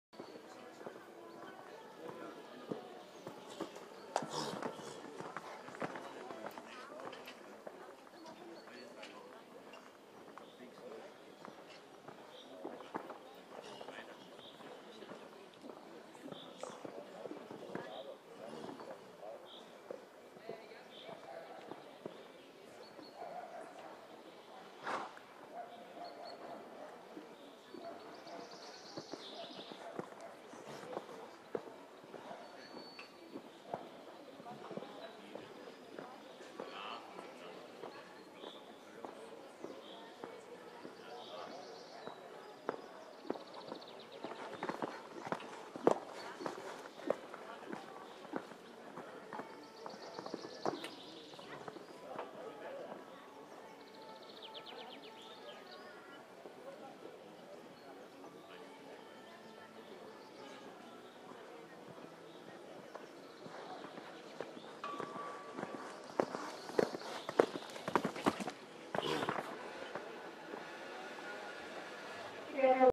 2013 Zangersheide top jumping gelding 135 cm / 140 cm level
Langbeiniger Sportler mit extrem viel Vermögen, bester Einstellung, Vorsicht und einfachem Handling. SPF M hocherfolgreich.

Long legged sport Zangersheide gelding with easiest handling, great carefulness and endless potential. Several times 125 cm placed.